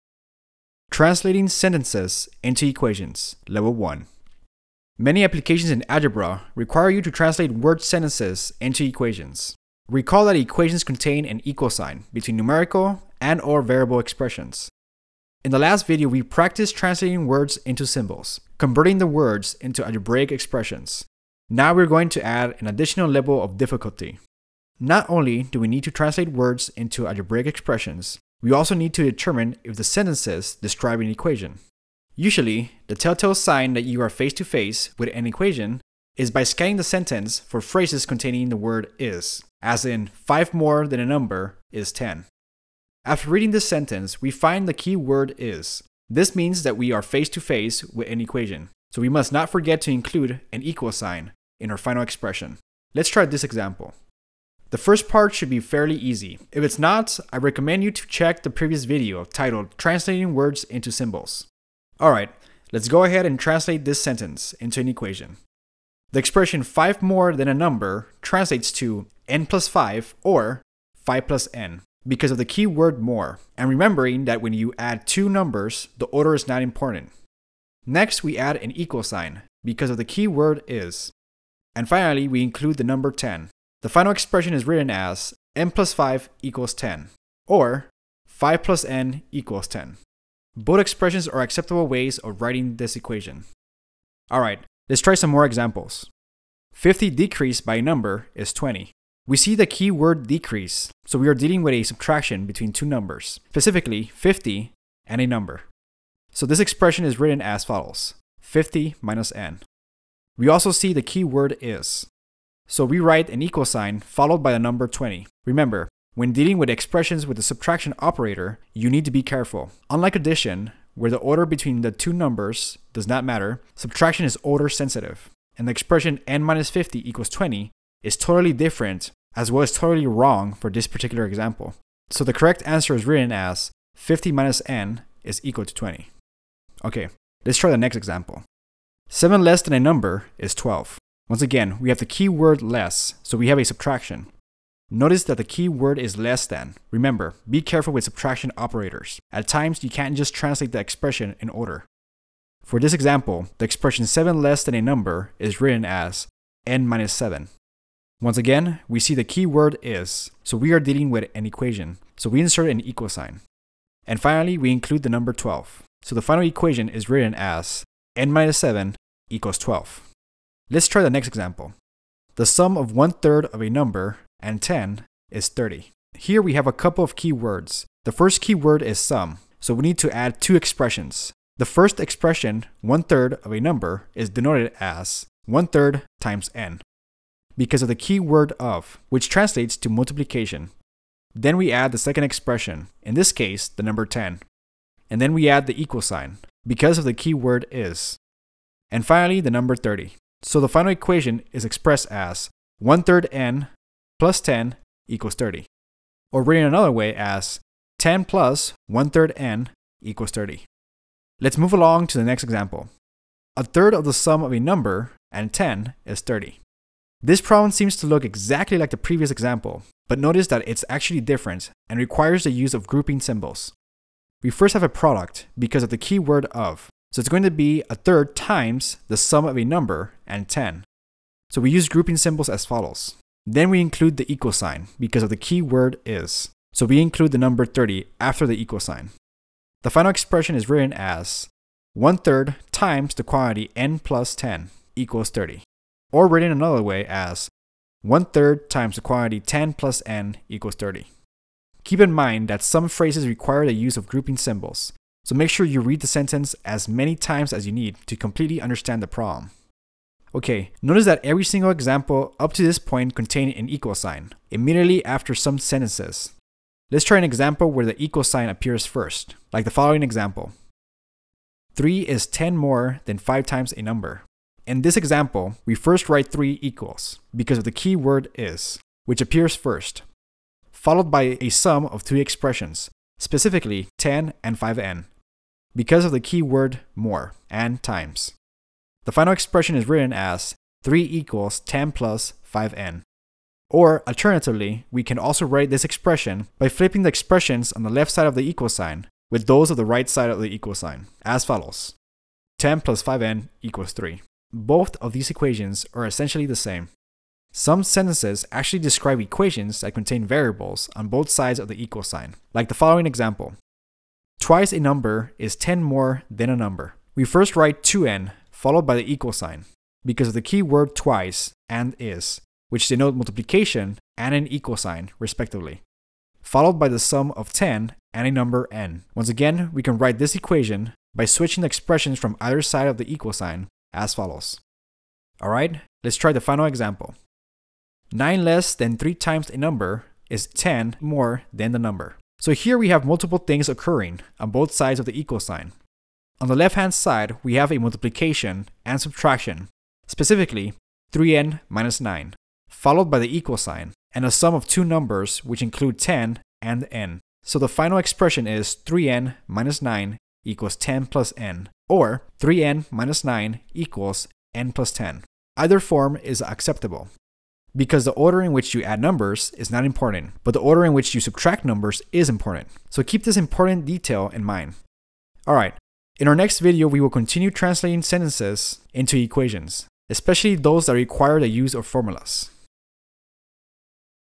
Algebra I: Translating Sentences into Equations (Level 1 of 2) | Examples I
This video goes over 8 examples, covering the proper way to translate sentences usually containing the key word "is" into equations.

0:00 Introduction
0:52 Example 1
1:35 Example 2
2:18 Example 3
2:51 Example 4
3:32 Example 5
4:30 Example 6
5:11 Example 7
5:36 Example 8